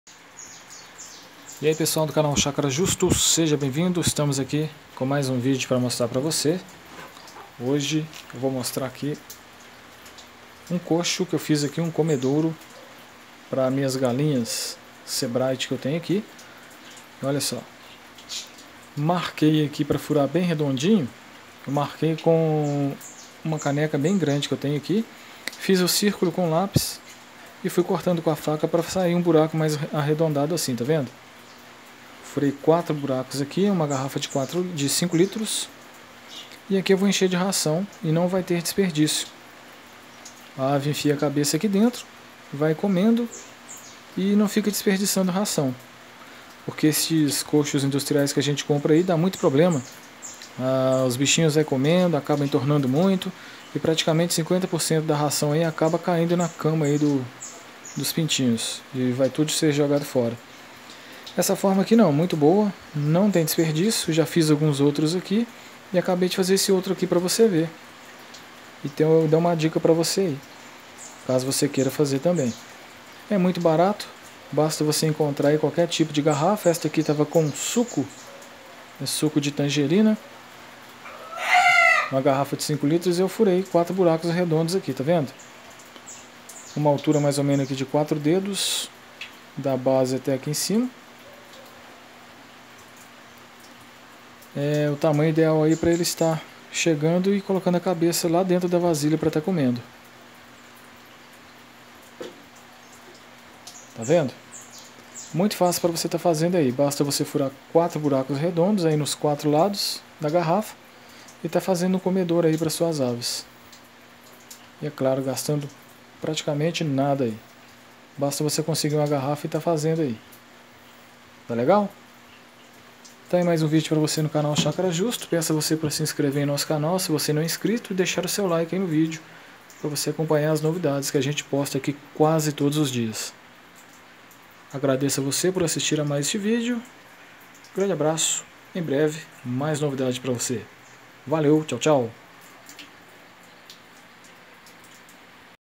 Dica: fazer um comedouro caseiro simples fácil para Galinhas pintos frangos gastando pouco dinheiro
. CONFIRA O NOSSO TIKTOK

Dica para fazer um comedouro simples e fácil para Galinhas pintos e frangos gastando pouco dinheiro